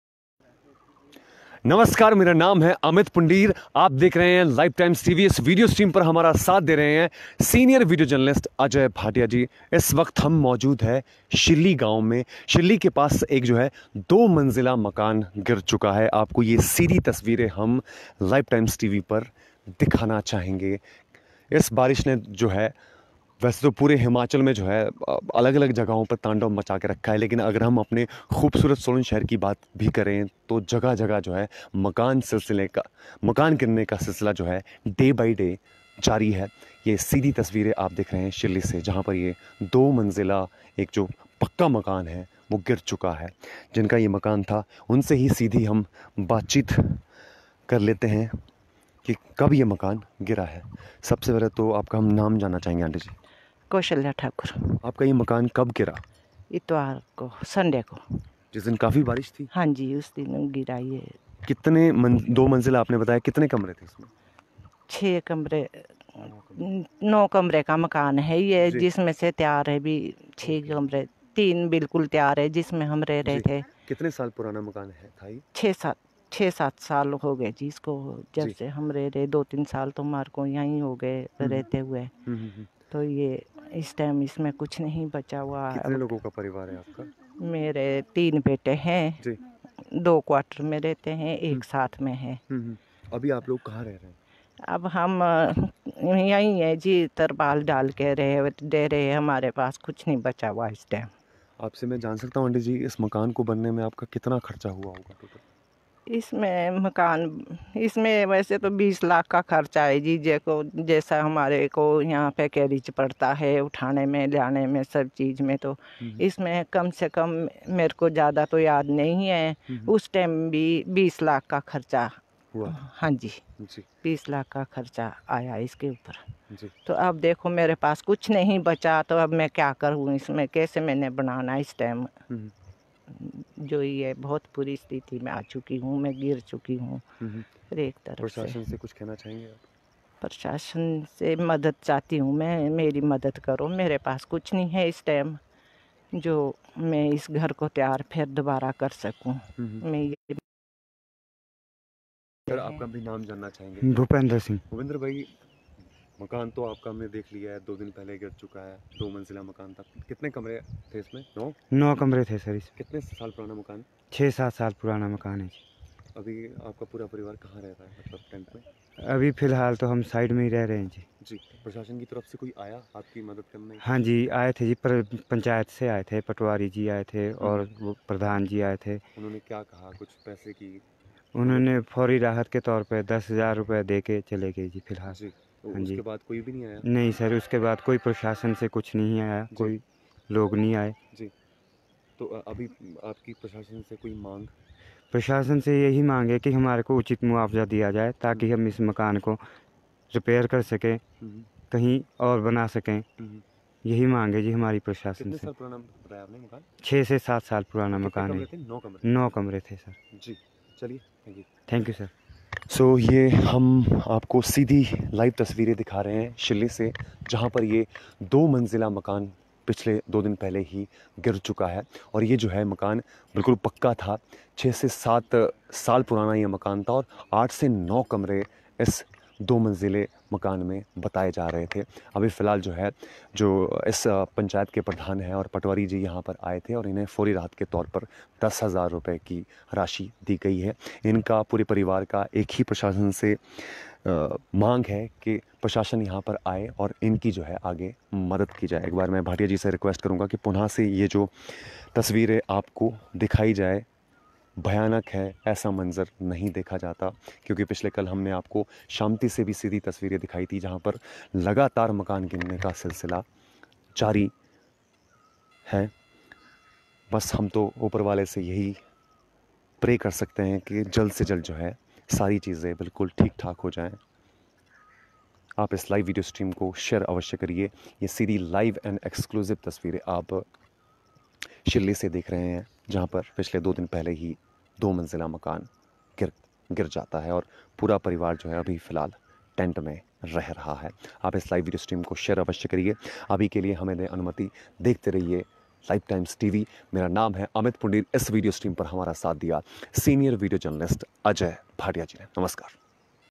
सोलन लाइव: शिल्ली में गिरा दो मंजिला मकान।
Solan Live: Two storey house collapsed in Shilli.